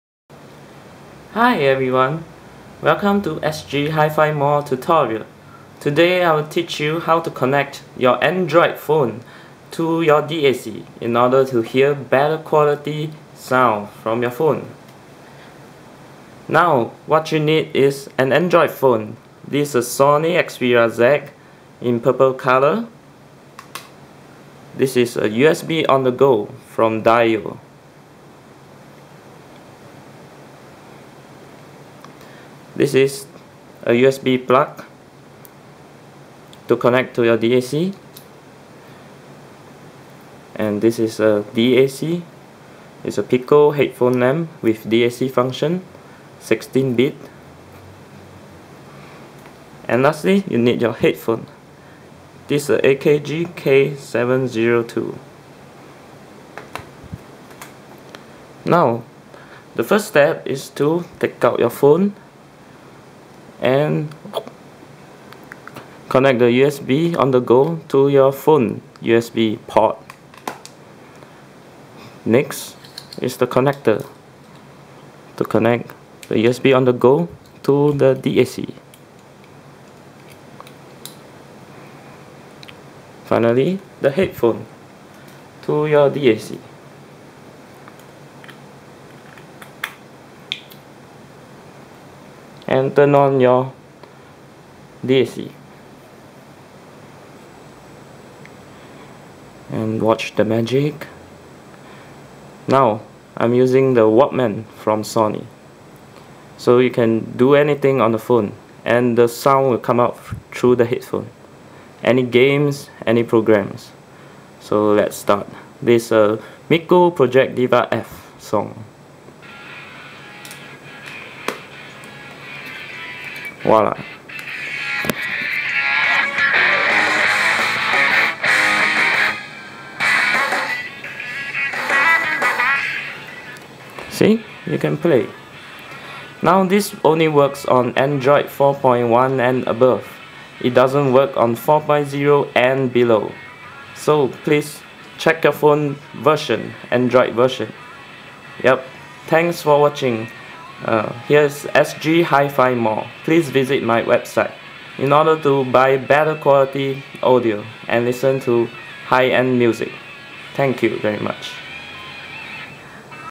(Tutorial) Connecting Android Devices to DAC (Xperia Z, Headamp Pico Amp+DAC, USB OTG)
for more hifi equipments!

This is the first SGHIFIMALL tutorial on connecting your android device to DAC.

Things needed: Android phone (4.1 & above), USB on-the-go adapter(USB OTG) , USB cable, DAC, Headphone/Earphone/Speaker

1. Connect USB OTG to the phone USB port
2. Connect USB cable to the USB OTG
3. Connect the USB cable to the DAC
4. Connect the Headphone/Earphone/Speaker to the DAC

Now Enjoy your games, music, movies with better quality sound than straight from your phone! If you use a portable dac like mine, you can listen to high end audio on the go! You wont be missing your high end audio at home so much when you travel overseas :)